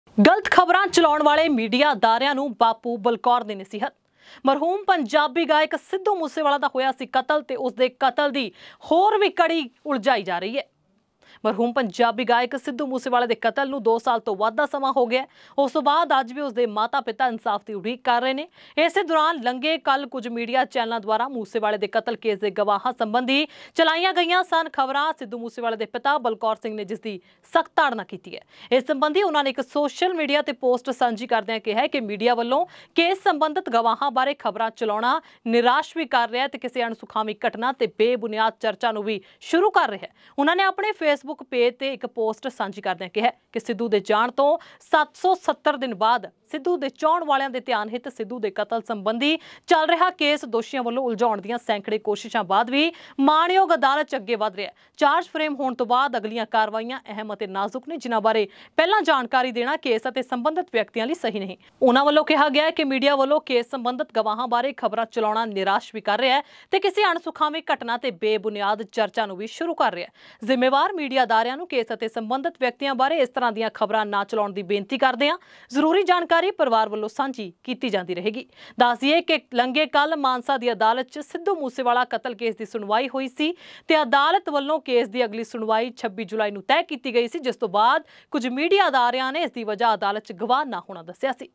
ਗਲਤ ਖ਼ਬਰਾਂ ਚਲਾਉਣ ਵਾਲੇ Media ਅਦਾਰਿਆਂ ਨੂੰ ਬਾਪੂ ਬਲਕੌਰ ਦੀ ਨਸੀਹਤ | Sardar Balkaur Singh Advice
ਗਲਤ ਖ਼ਬਰਾਂ ਚਲਾਉਣ ਵਾਲੇ Media ਅਦਾਰਿਆਂ ਨੂੰ ਬਾਪੂ ਬਲਕੌਰ ਦੀ ਨਸੀਹਤ | Sardar Balkaur Singh Advice | Punjab News | Sanjhi Soch Tv